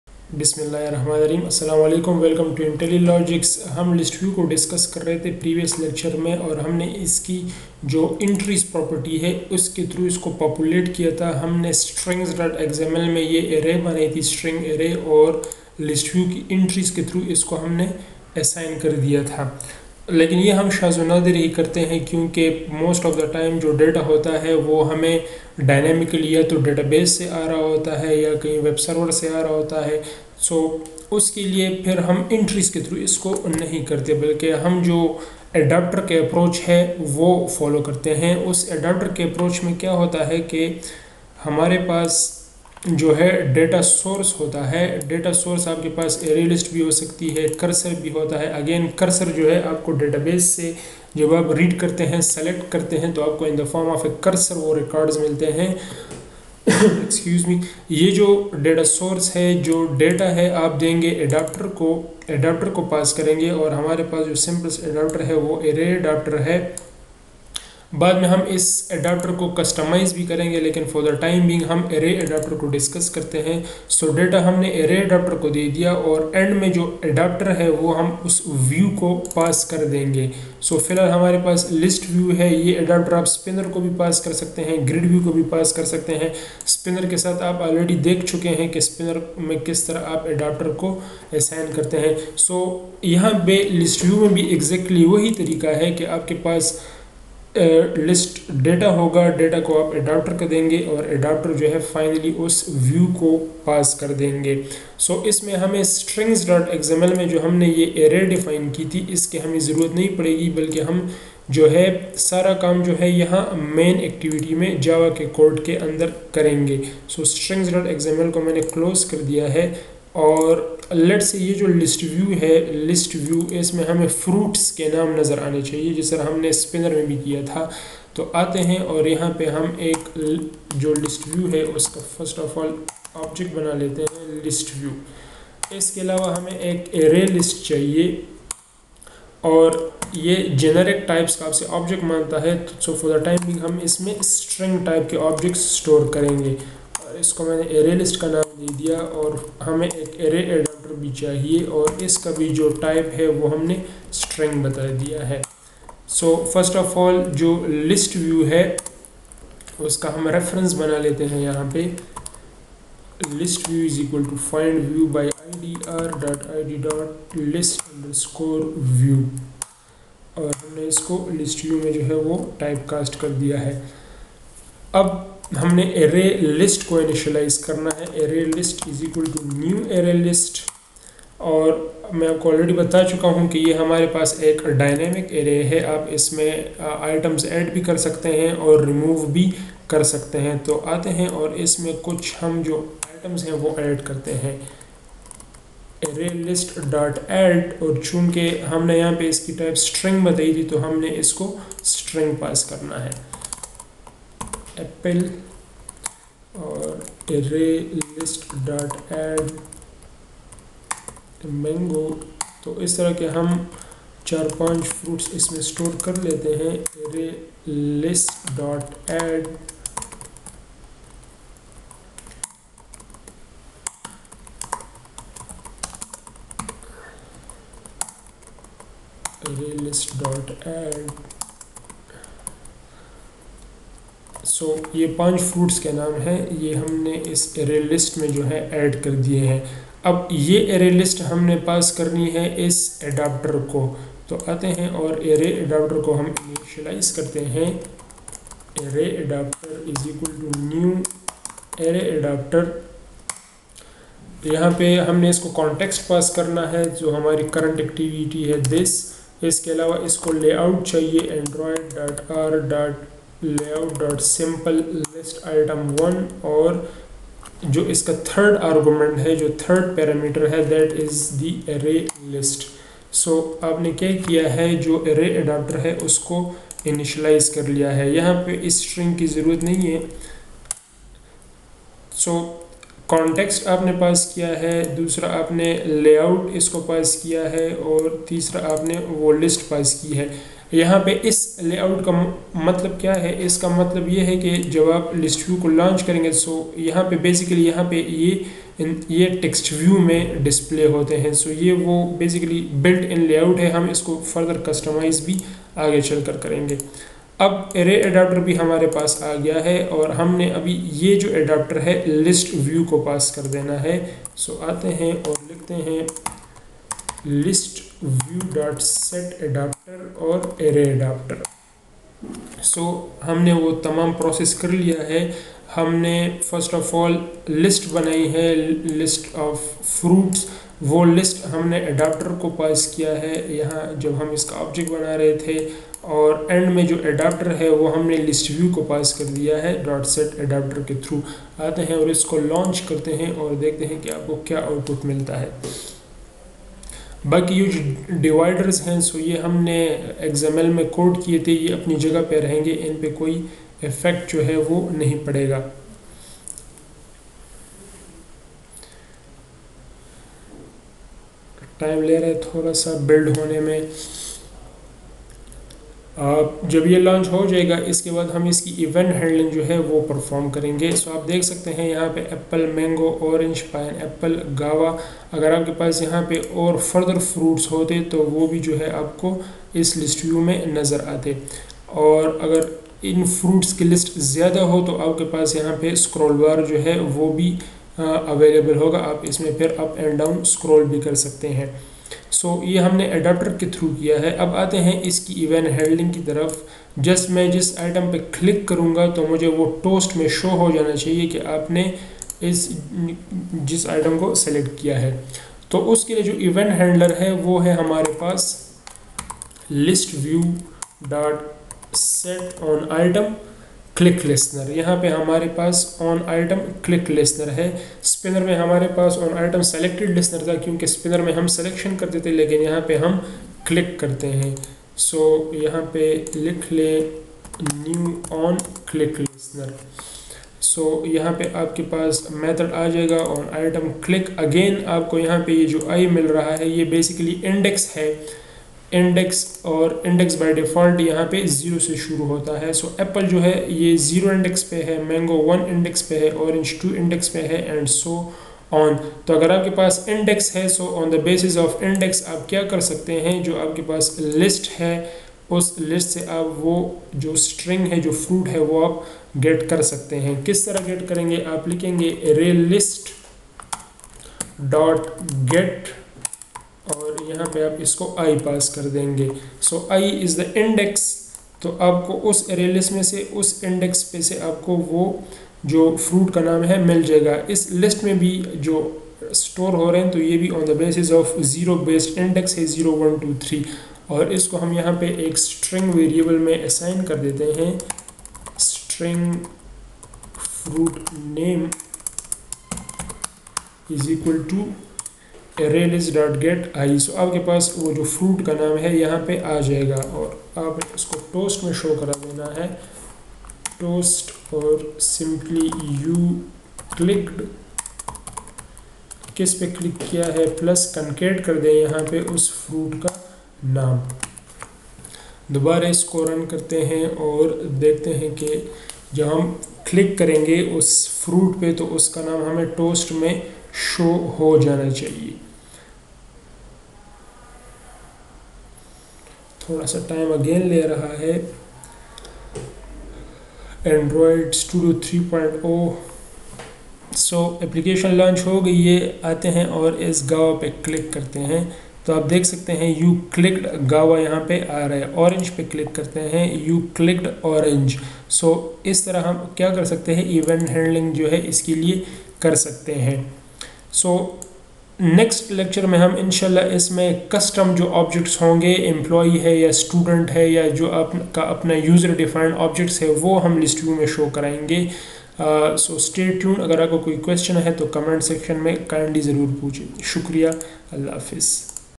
38. ListView Part 2 with ArrayAdapter, OnItemClickListener Urdu/Hindi
Video shows how to use ListView in Android.
Different terms related to ListView are discussed such as
ListView
ArrayAdapter
ArrayList
OnItemClickListener